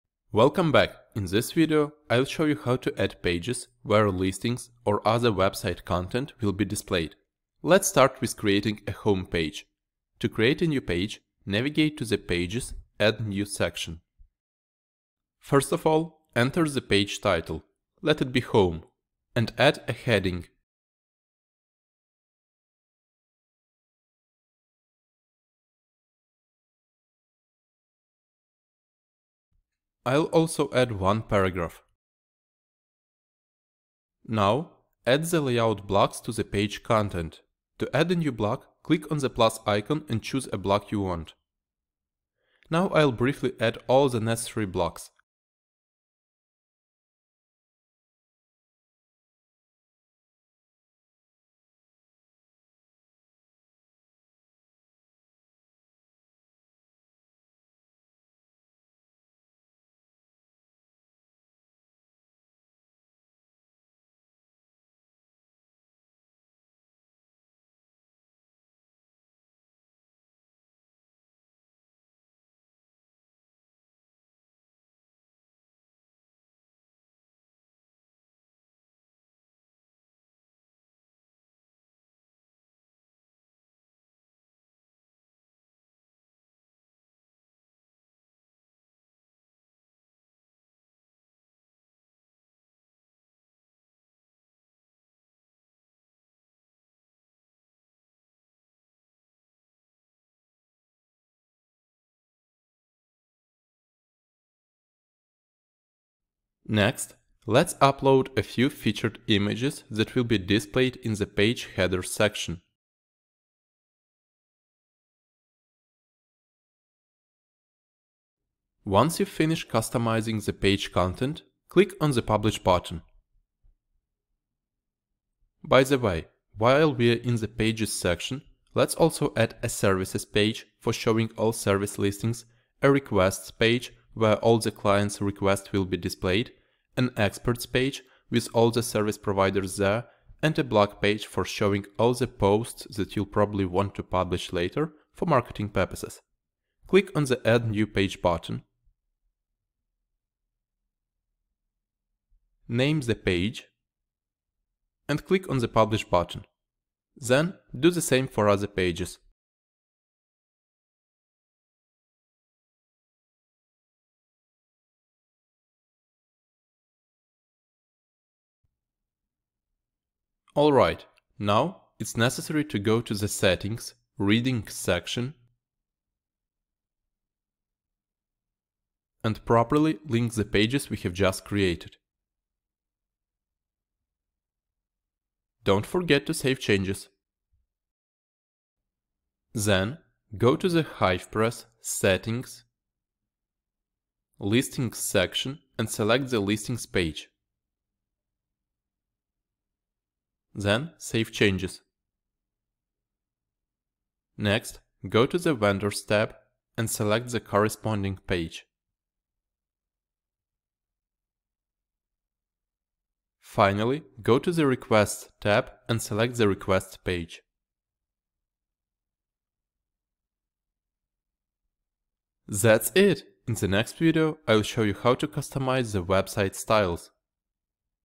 Adding Pages [ExpertHive Course - 6/11]
This is one of the videos from our course on how to create a service marketplace with WordPress.